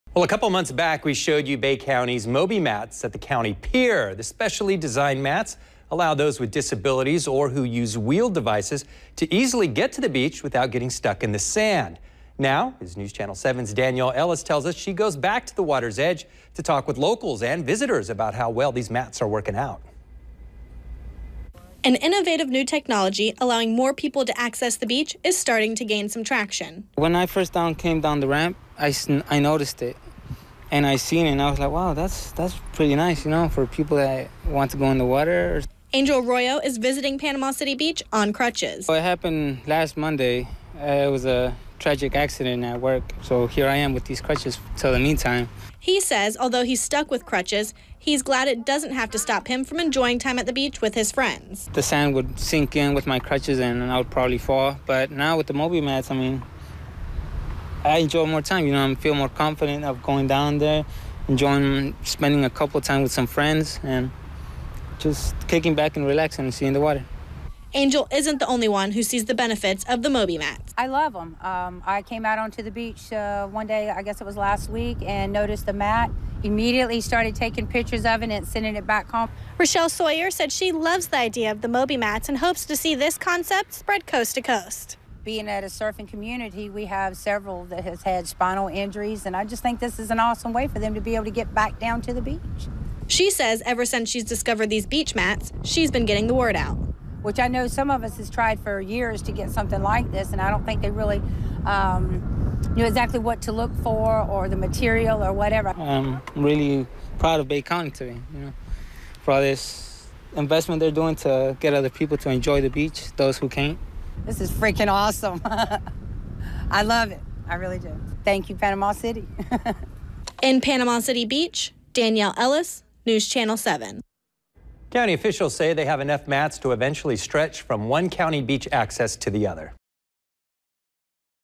Mobi-mat® - Gaining traction with locals and visitors at Panama City Beach, Florida, USA
"A couple months back, we showed you Bay County Mobi-mat® at the county pier, the specially designed mats allow those with disabilities or who use wheel devicsto easily get to th beach without getting stuck in the sand.
Now this news channel 7 Daniel Ellis, tell us she goes back to the water's edge to talk with locals and visitors about how well this mats are working out!

An innovative new technology allowing more people to access the beach is starting to gain some traction.
- When I first came down the ramp I noticed it and I seen it and was like wow, that's pretty nice you know for people that want to go in the water.

Angel Arroyo is visiting Panama City Beach on crutches.
- What happened last Monday, with a tragic accident at work so here I am with these crutchesuntil the meantime.

He says although, he's stuck with crutches, he's glad he doesn't have to stop him enjoying time at the beach with his friends.
- The sand would sink in with my crutches and I would probably fall but now with Mobi-mat® I enjoy more time, I'm feeling more confident of going down there, enjoying spending a couple times with some friends and just kicking back and relax in the water.

Angel is not the only one who see's the benefit of the Mobi-mat®.
- I love them, I came out to the beach, one day I guess it was last weekand noticed the mat, immediately started taking pictures of them and send it back home.

Rachelle Sawyer said she loves the idea of Moi-mat® and hpes to see these concept spread coast-to-coast.
- Being at a certain community, we have several that has spinal injuries and I just think this is an awesome way for them to be able to get back down to the beach.

She says ever since she discovered these beach mats she's been getting the word out.
- Which I know some of us has tried for years to get something like this and I don't think they really knew exactly what to look for, the material or whatever.
- I'm really poud of the country for this investment they're doing to get the people to enjoy the beach, those who can't.
- This is freaking awesome ! I love it, I really do ! Thank you Panama City !"